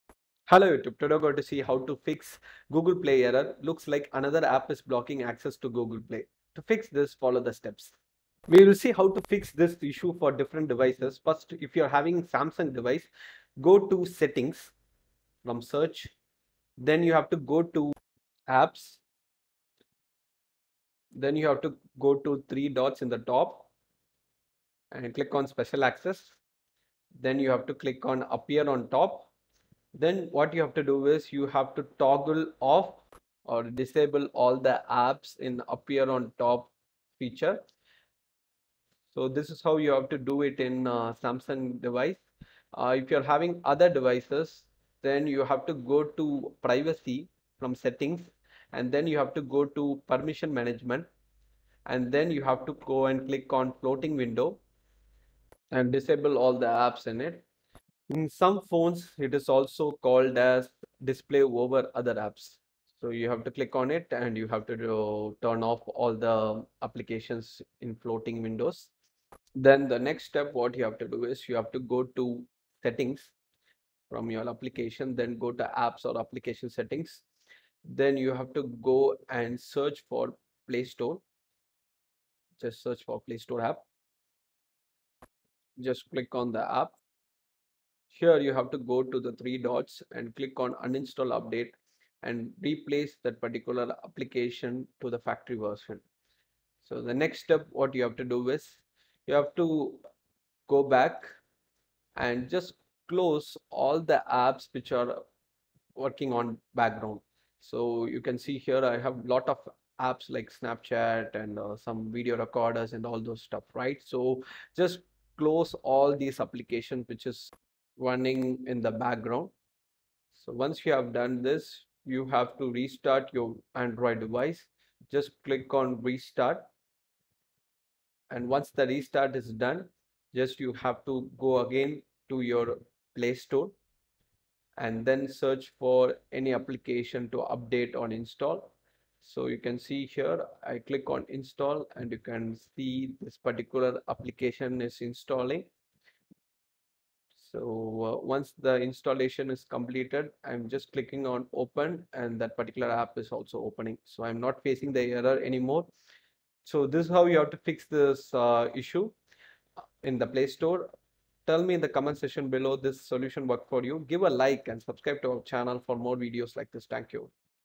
Looks like another app is blocking access to google play Turn off any apps Problem Fix
This video solved Google play store Problem looks like another app is blocking access to google play pay in Samsung , oppo , one plus ,vivo, realme, mi phones Turn off any apps that might be drawing over the screen and try again looks like another app is blocking google play